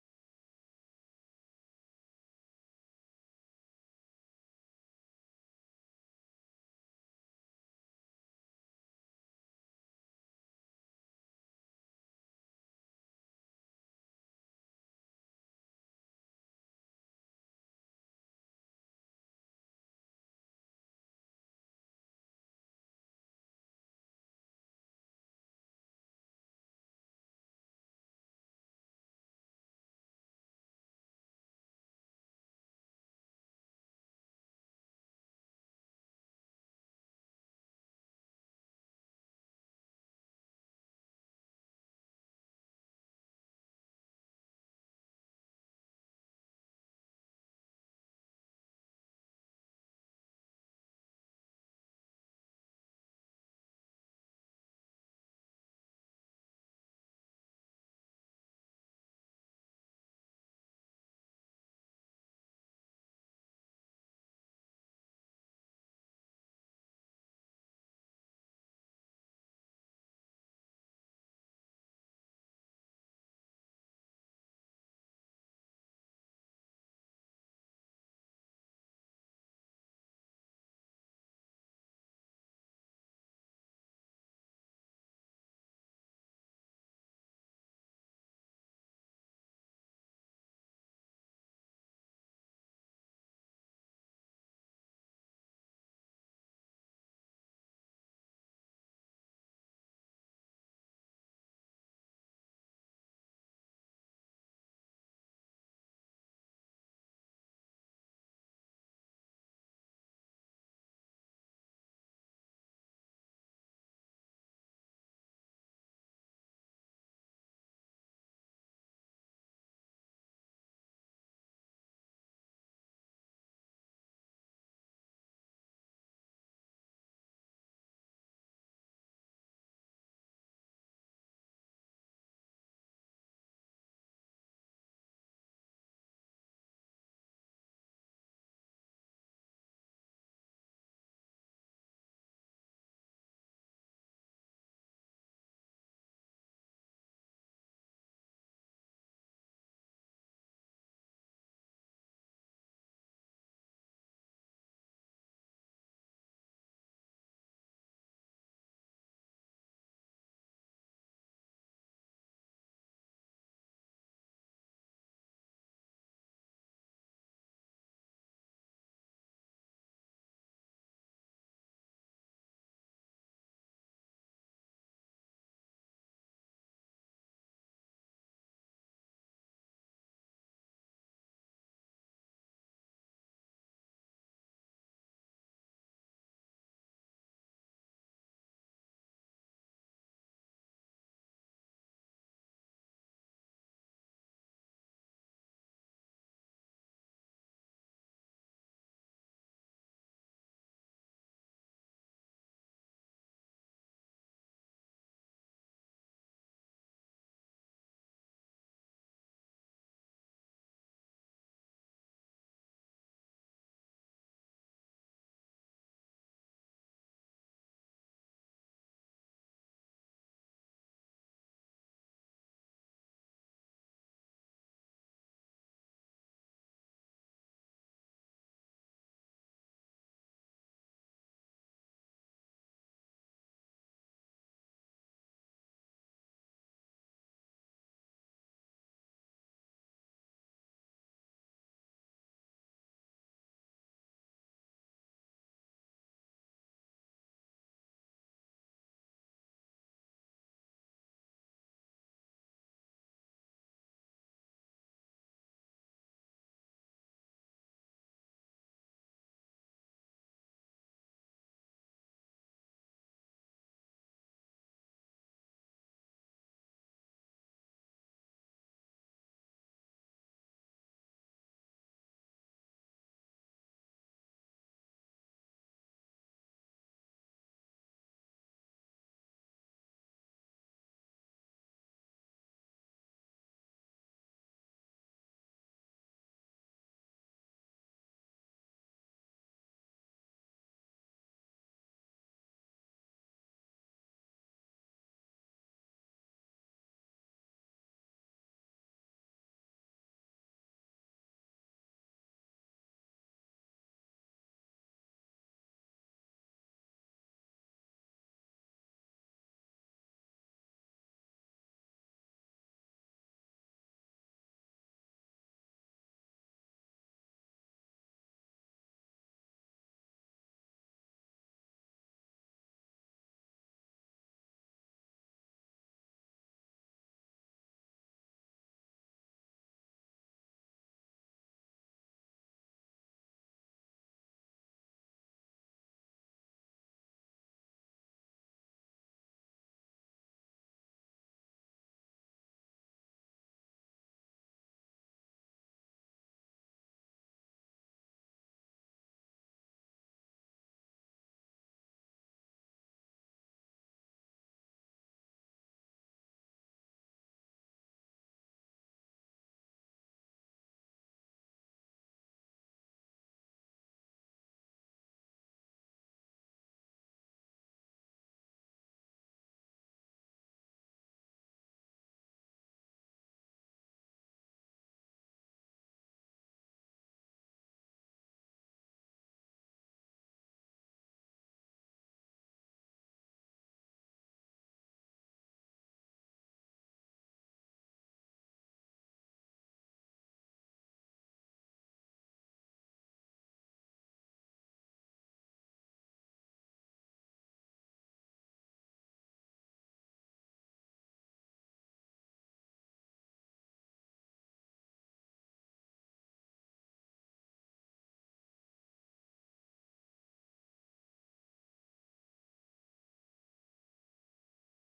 Create technical illustrations from CAD #1
Process for creating detailed technical illustrations from CAD.